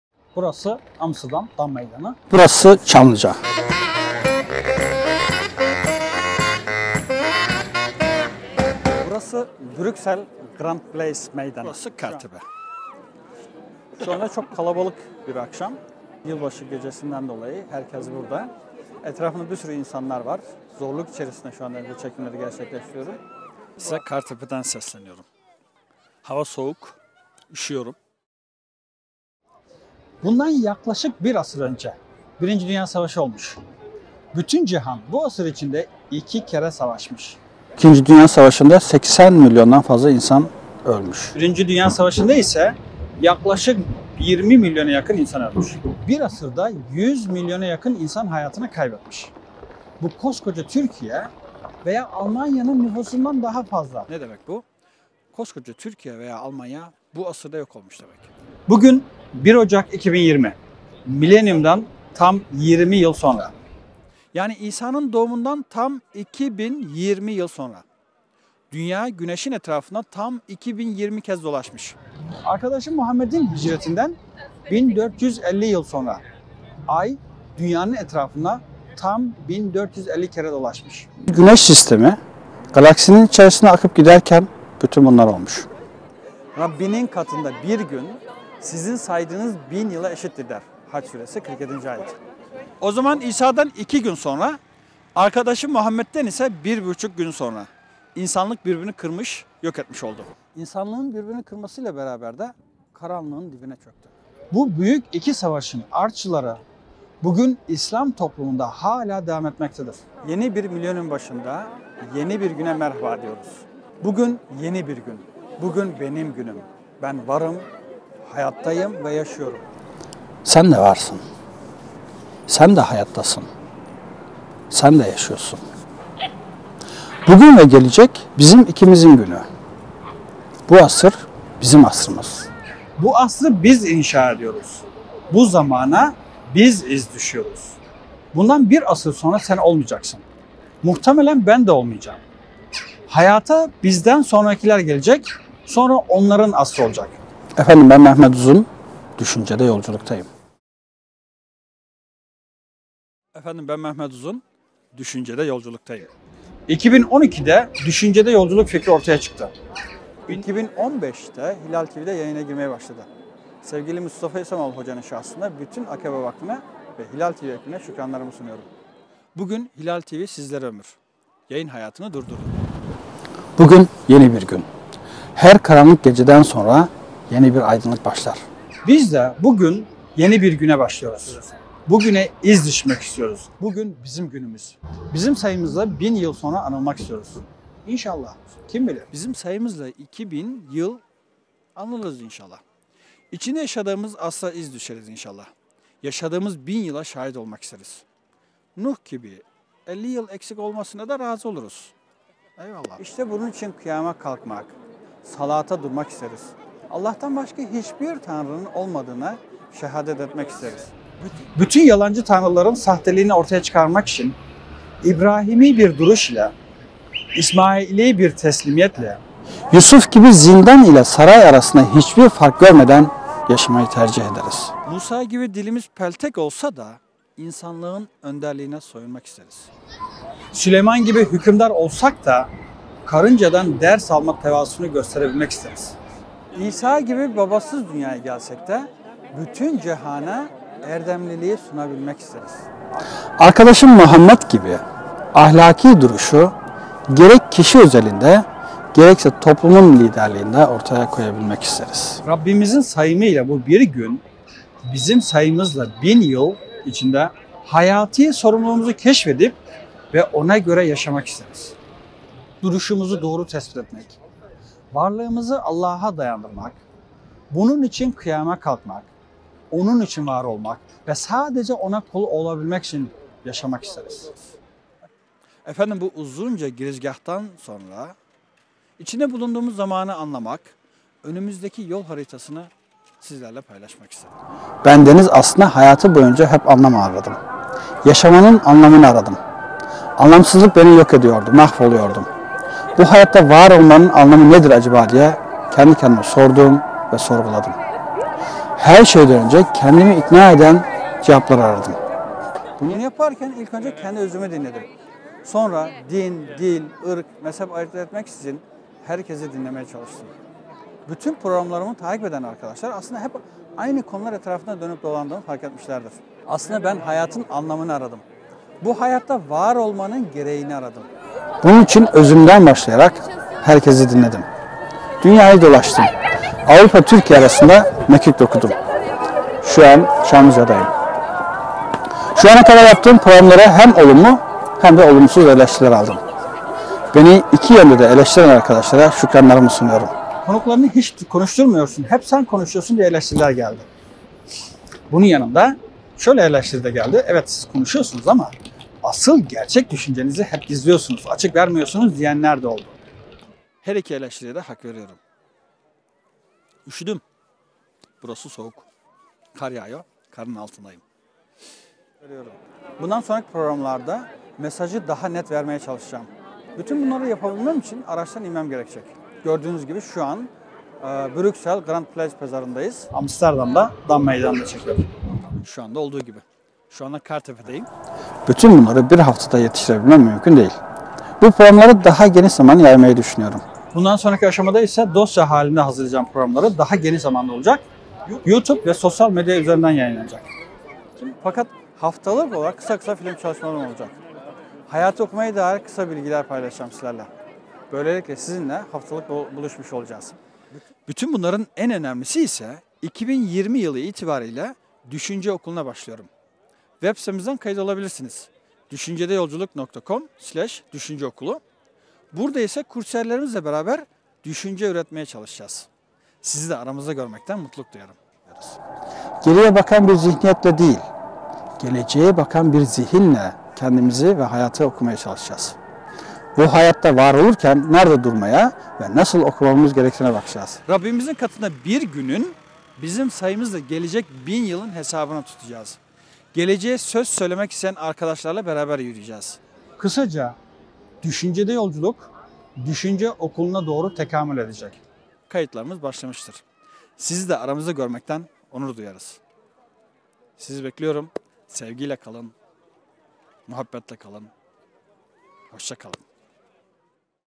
DY 2020 - 00 Giriş
Bundan yaklaşık bir asır önce birinci dünya savaşı olmuş. Bütün cihan bu asır içinde iki kere savaşmış. İkinci dünya savaşında 80 milyondan fazla insan ölmüş. Birinci dünya savaşında ise yaklaşık 20 milyon insan ölmüş. Bir asırda 100 milyona yakın insan hayatını kaybetmiş. Bu koskoca Türkiye veya Almanya'nın nüfusundan daha fazla. Bu ne demek : koskoca Türkiye veya Almanya yok olmuş demek.

Bugün bir ocak 2020. Millennium dan 20 yıl sonra.

Yani İsa'nın doğumundan tam 2 bin 20 yıl sonra. Dünya güneşin etrafında tam 2 bin 20 kez dolaşmış. Arkadaşım Muhammed'in hicretinden 1450 yıl sonra. Ay dünyanın etrafında tam 1450 kere dolanmış. İçinde yaşadığımız güneş sistemi galaximizin içinde akıp giderken bunlar olmuş.

"Rabb'inin katında bir gün, sizin saydığınız bin yıla eşittir" der haç suresi 47'ci ayet.

O zaman İsa'dan iki gün sonra, arkadaşım Muhammed den ise bir buçuk gün sonra insanlık birbirini kırmış yok etmiş oldu. İnsanlığın birbirini kırmasıyla beraber de  karanlığın dibine çöktü.
Bu büyük iki savaşın artçıları bu gün İslam toplumlarında hala devam ediyor.
Yeni bir millennium başında yeni bir güne merhaba diyoruz.
Bu gün yeni bir gün. Bu gün benim günüm. Ben varım. Hayattayım ve yaşıyorum.
Sen de varsın. Sen de hayattasın. Sen de yaşıyorsun. Bugün ve gelecek bizim ikimizin günü. Bu Asır bizim asrımız. Bu asrı biz inşa ediyoruz. Bu zamana biz iz düşüyoruz.
Bundan bir asır sonra sen de olmayacaksın, ben de olmayacağım. Hayata bizden sonrakiler gelecek. Sonra onların asrı olacak.
Efendim, ben Mehmet Uzun,
Düşüncede Yolculuk dayım..
2012'de Düşüncede Yolculuk fikri ortaya çıktı. 2015’te Hilal tv'de yayına girmeye başladı. Sevgili Mustafa İslamoğlu hocanın şahsında bütün Akabe vakfına, ve Hilal TV ekibine teşekkürlerimi sunuyorum. Bugün Hilal tv sizlere ömür. Yayını hayatını durdurdu.
Bu gün yeni bir gün. Her karanlık geceden sonra yeni bir aydınlık başlar. Biz de bu gün yeni bir güne başlıyoruz ve bugüne biz iz düşmek istiyoruz. Bugün bizim günümüz. Bizim sayımızla 2 bin yıl sonra anılmak istiyoruz, inşaAllah.
İçinde yaşadığımız asra iz düşeriz inşaAllah. Yaşadığımız bin yıla şahit olmak isteriz. Nuh gibi bin yıldan 50 yılın eksik olmasına da razı oluruz, EyvaAllah.
İşte bunun için kıyama kalkmak, salata durmak isteriz. Allah'tan başka hiçbir tanrının olmadığına şehadet etmek isteriz.
Bütün yalancı tanrıların sahteliğini ortaya koymak için İbrahimi bir duruş ile, İsmail'i bir teslimiyetle, Yusuf gibi zindan ile saray arasında hiçbir fark görmeden yaşamayı tercih ederiz.
Musa gibi dilimiz peltek olsa da insanlığın önderliğine soyunmak isteriz..
Süleyman gibi hükümdar olsak da karıncadan ders alma tevazusunu gösterebilmek isteriz.
İsa gibi babasız dünyaya gelsek de bütün cihana erdemliliği sunmak isteriz.
Arkadaşım Muhammed gibi ahlaki duruşu gerek kişi özelinde gerekse bir toplumun liderliğinde olarak ortaya koyabilmek isteriz.
Rabb'imizin sayımıyla bu bir gün, bizim saymamız ile bin yıl içinde hayati sorumluluğumuzu keşfetmek ve ona göre yaşamak isteriz.
Duruşumuzu doğru tespit etmek. Varlığımızı Allah'a dayandırmak. Bunun için kıyama kalkmak. O'nun için var olmak ve sadece O'na kul olmak için yaşamak isteriz.
Efendim,
Bu uzunca girizgahtan sonra içinde bulunduğumuz zamanı anlamak ve önümüzdeki yol haritasını sizlerle paylaşmak istedim.
Ben deniz aslında hayatım boyunca hep anlam aradım. Yaşamanın anlamını aradım. Anlamsızlık beni yok ediyordu. Bu hayatta var olmanın anlamı nedir acaba diye kendi kendime sordum, sorguladım. Her şeyden önce kendimi ikna eden cevaplar aradım.
Bunu yaparken ilk önce kendi özümü dinledim. Sonra din, dil, mezhep, ırk ayırt etmeksizin herkesi dinlemeye çalıştım. Bütün programlarımı takip eden arkadaşlar aslında hep aynı konular etrafında dönüp dolandığımı fark etmişlerdir. Aslında ben hayatın anlamını aradım. Bu hayatta var olmanın değerini aradım. Bunun için özümden başlayarak herkesi dinledim. Dünyayı dolaşmaya çalıştım. Avrupa Türkiye arasında mekik dokudum.
Şu ana kadar yaptığım programlara hem olumlu hem olumsuz eleştiriler aldım. Beni iki yönde de eleştiren arkadaşlara şükranlarımı sunuyorum.
Hayatımda yeni bir gün başlıyor. Bu yeni dönemde şu şekilde çalışmaya karar verdim.
Düşüncede Yolculuk sadece sohbet etmenin ötesine geçmeli. Seyirciye sonuç bir fikir vermeli. Şu ana kadar konuklarımla aracın içinde sohbet etmiştik. Sohbet esnasında ben çok konuşsam da aslında kendimi tutmuş konuklarımın konuşmasına fırsat vermiştim.
'Konuklarını hiç konuşturmuyorsun, hep sen konuşuyorsun' diye bana çok eleştiriler geldi. Bunun yanında şöyle de eleştirenler oldu: "evet, siz konuşuyorsunuz, ama asıl gerçek düşüncelerinizi hep gizliyorsunuz. Açık vermiyorsunuz" diyenler de oldu.
Bu iki eleştiriye de hak veriyorum.